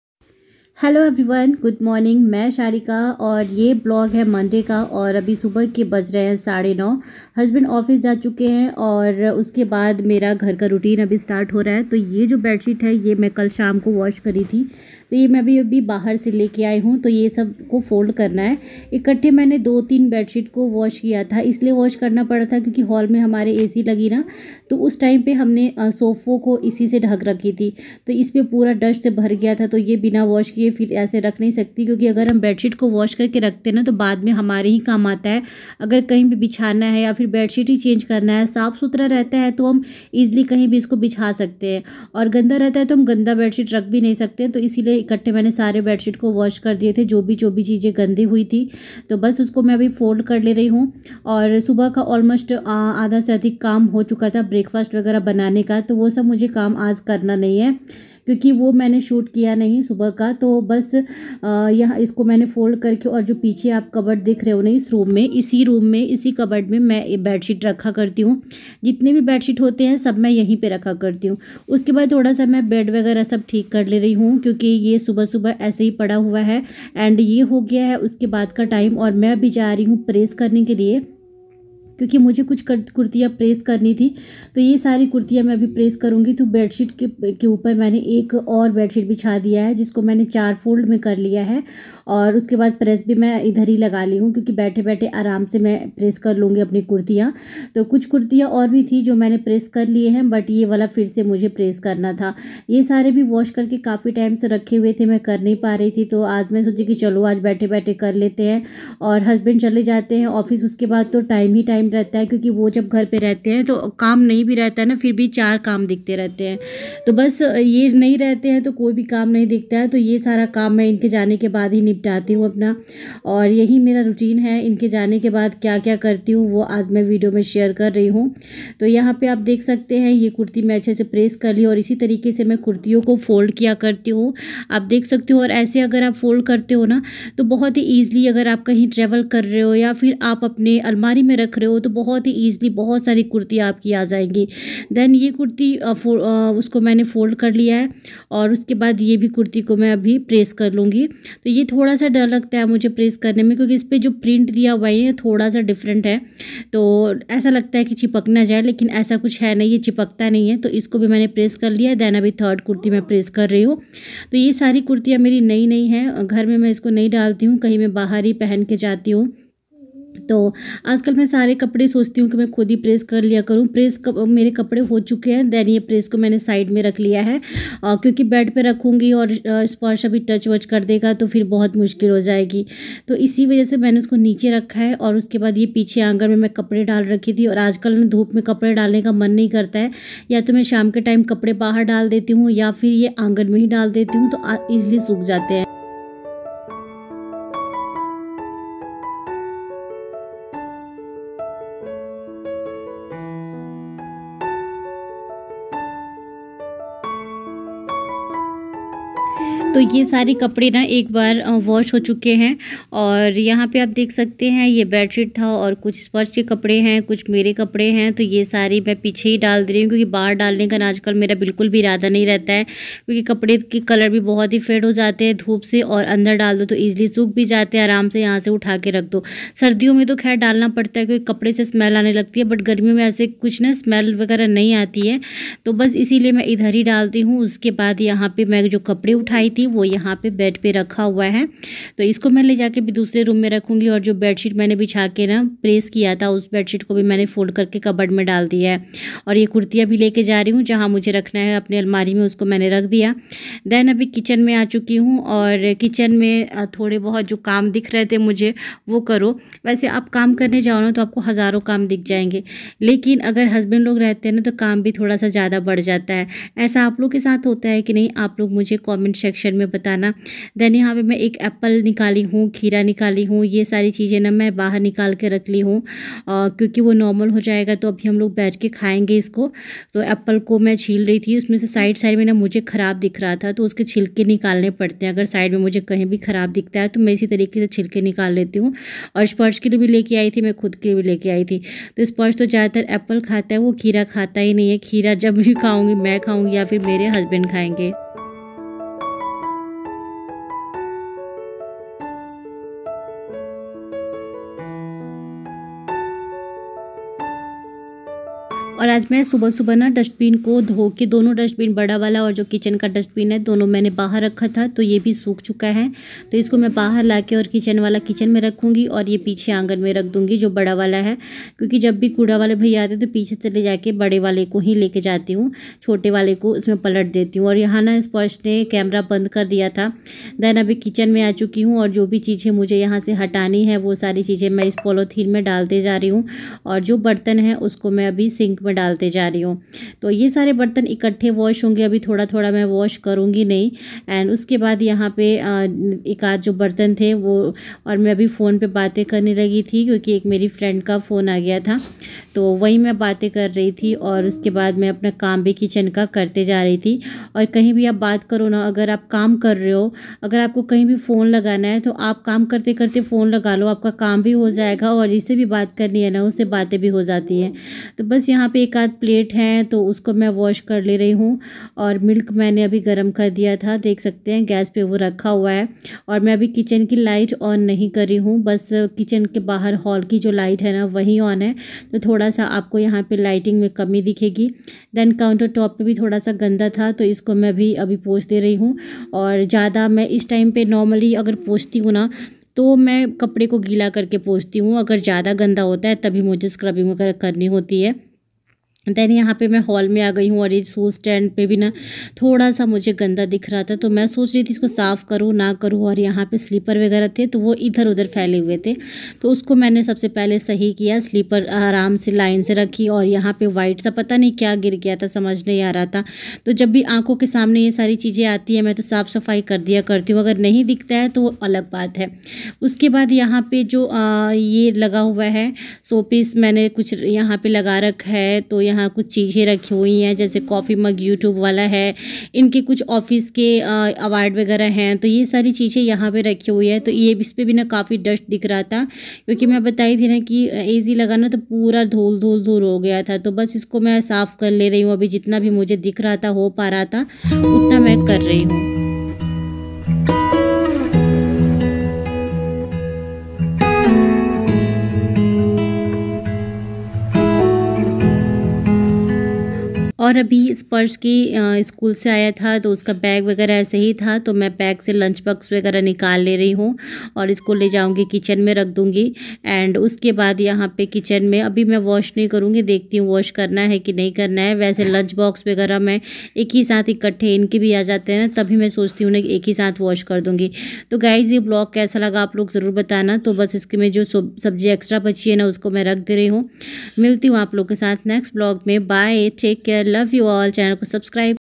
पति के जाने के बाद💁आज कैसे बीता घर में पूरा दिन/indian mom morning routine after the husband left
पति के जाने के बाद💁आज कैसे बीता घर में पूरा दिन 🏘️my early morning routine after the husband left